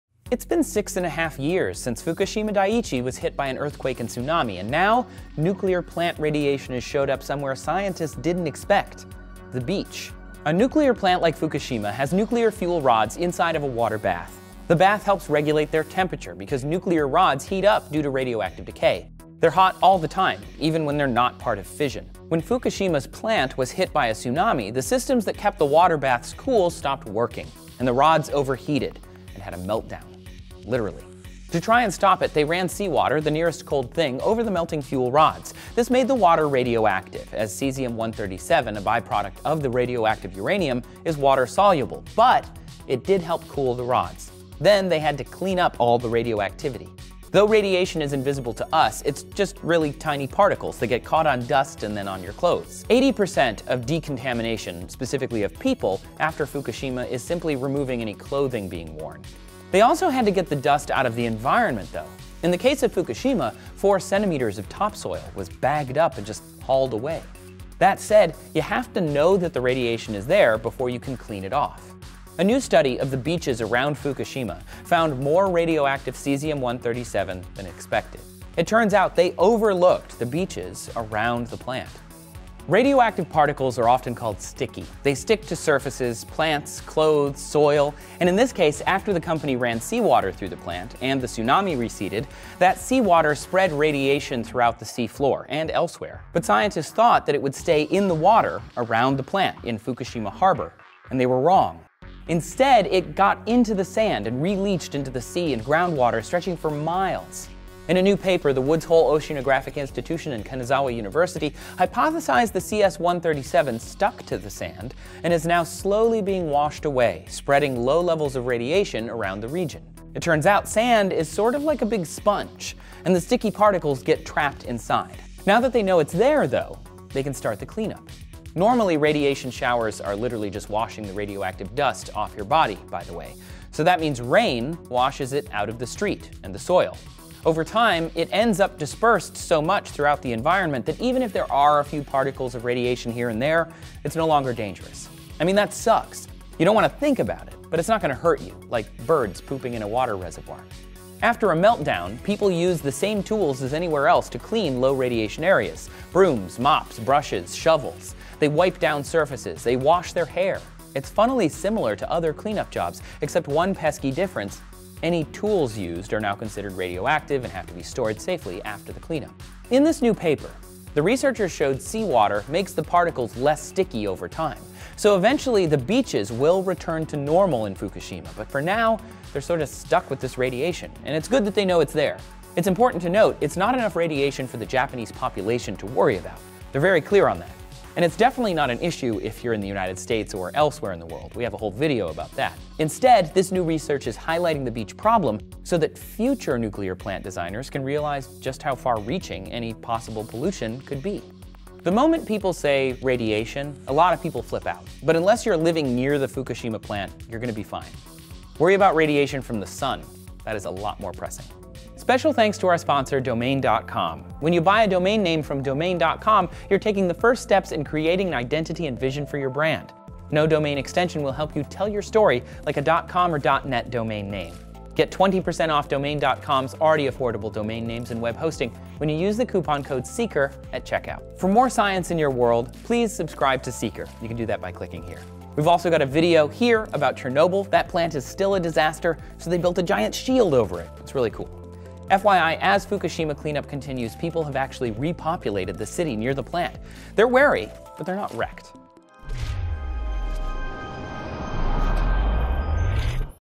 A New Source of Fukushima Radiation Was Just Found, Now What?
Researchers found radioactive particles from Fukushima on beaches miles away, but how did it get there? And should we be worried?

Scientists find new source of radioactivity from Fukushima disaster
"Scientists have found a previously unsuspected place where radioactive material from the Fukushima Dai-ichi nuclear power plant disaster has accumulated-in sands and brackish groundwater beneath beaches up to 60 miles away. The sands took up and retained radioactive cesium originating from the disaster in 2011 and have been slowly releasing it back to the ocean."

What Does Radiation Poisoning Do to Your Body?
"There are a few kinds of radiation. Some is light or heat, but the kind that damages tissue is called ionizing radiation.  Ionizing radiation comes in high-energy waves like x-rays and gamma rays, or from nuclear material via particle decay. This kind of radiation can damage atoms, which can affect whole molecules (like DNA), and that can affect your cells, organs and whole body."

Giant Robotic Mech Suit Is Said to Help Clean Up Fukushima
"Bulgarov is well known in video game and film circles for his skill designing futuristic robots for games like World Of Warcraft and Diablo 3 and movies including Robocop (2014), Transformers 4 and others. But this was something different. Bulgarov, used to rendering heroes and villains using 3-D computer modeling software, was invited by a Korean robotics company to build something real."

This episode of Seeker was written and hosted by Trace Dominguez